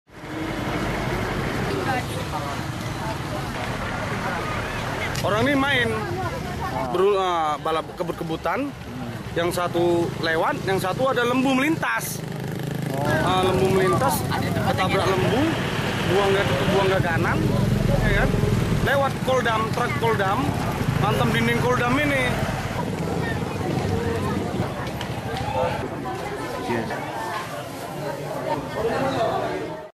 Minibus Tabrak Truk dan Sapi, 1 Tewas 7 Terluka
Metrotvnews.com, Langkat: Sebuah minibus L300 menabrak truk dan lembu di kawasan Binjai-Kuala, Kecamatan Selesai. Kabupaten Langkat, Sumatera Utara Rabu (8/1). Kecelakaan diduga sopir minibus melaju kencang hingga hilang kendali dan sapi milik warga yang berkeliaran di jalan. Satu orang tewas, dua penumpang kritis, lima lainnya luka ringan. Sementara itu pengemudi truk melarikan diri. Petugas Lantas Polsek Selesai masih mengidentifikasi kejadian tersebut dan melakukan pengejaran terhadap sopir truk. (Endang Junaidi)